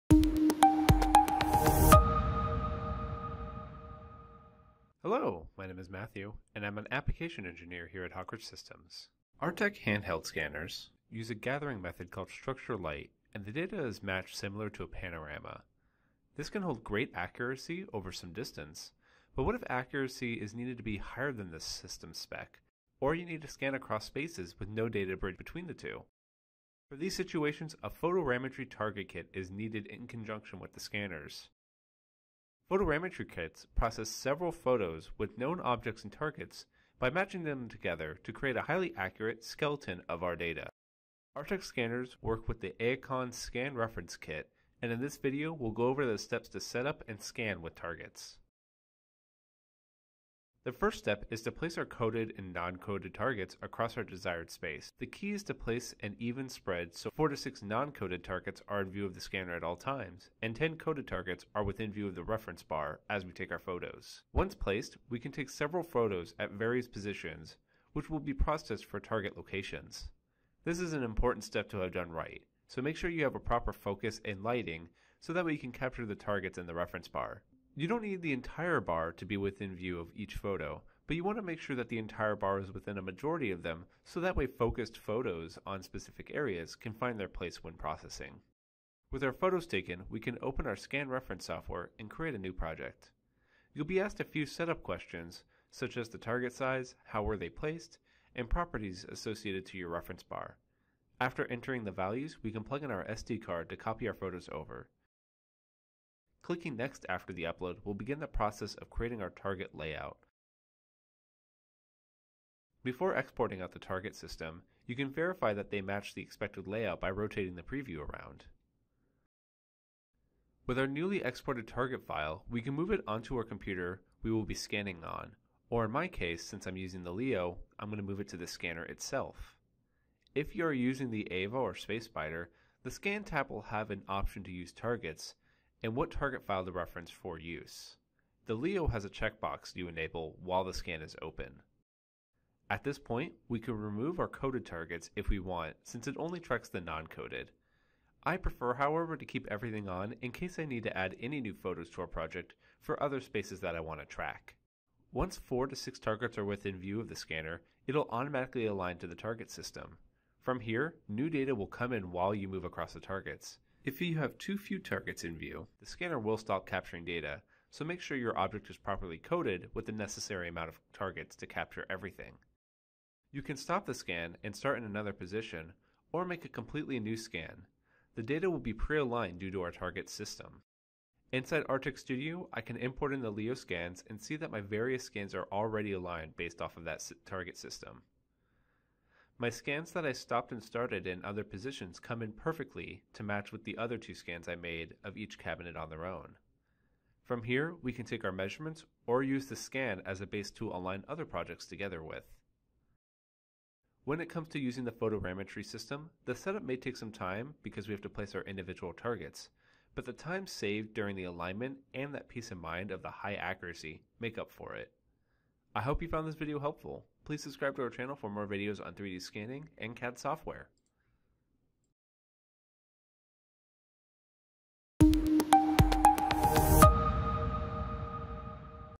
Using Photogrammetry with Artec 3D Scanners
Artec 3D Scanners are compatible with various 3rd party photogrammetry systems. These systems allow the Artec handheld scanners to achieve far greater repeatability and accuracy over distance than is possible with a standalone handheld scanner.